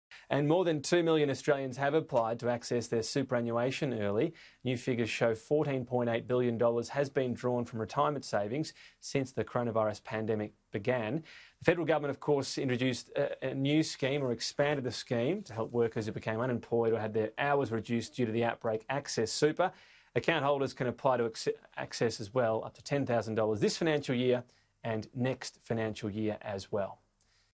$14.8bn withdrawn from superannuation
More than two million Australians have applied to access their superannuation early.

New figures show $14.8 billion has been withdrawn from retirement savings since the coronavirus pandemic began.

Image: Getty